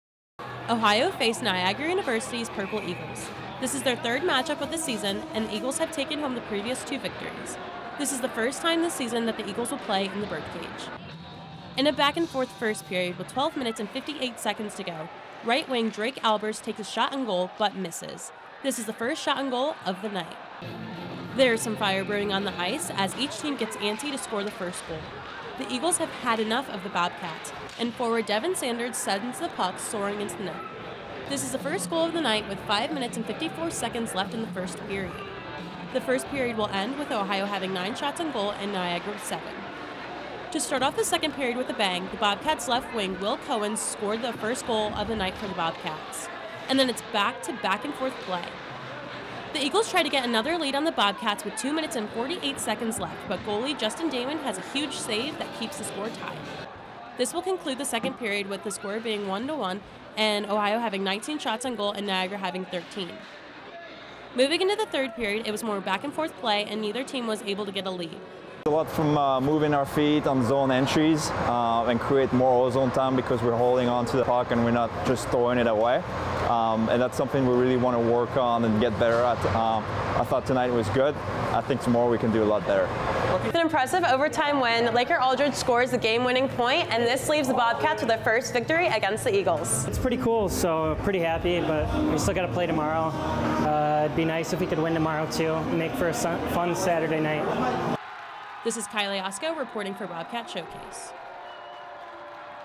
Ohio came away with a 2-1 overtime victory against Niagara
Ohio took down Niagara 2-1 in overtime

Laker Aldridge scored the game winning goal and says he hopes the team can get another win on Saturday night

🎥: Kyleigh Osko